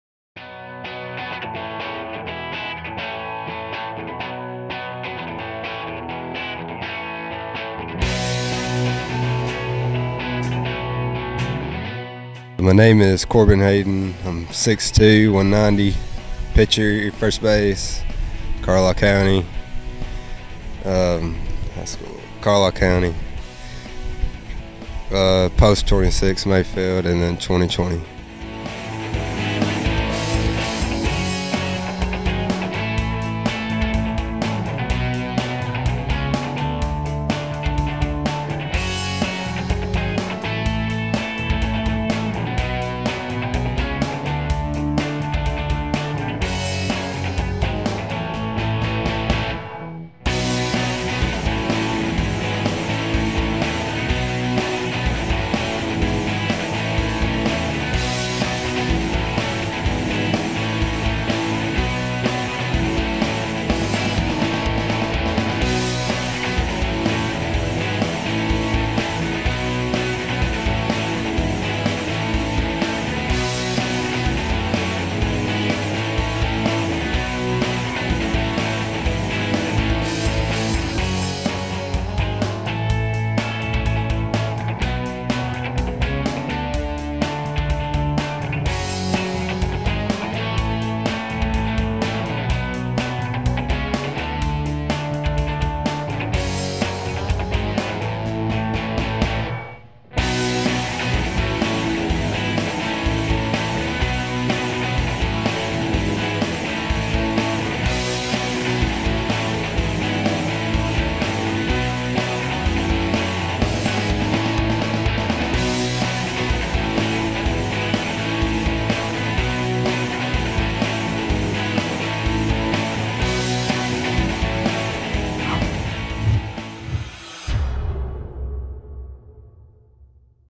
Corbin Hayden - LHP - Arlington, KY - 2020
All-Kentucky Crossroads Select Series filmed by SkillShow. Whitaker Park. Lexington, KY. July 17, 2018.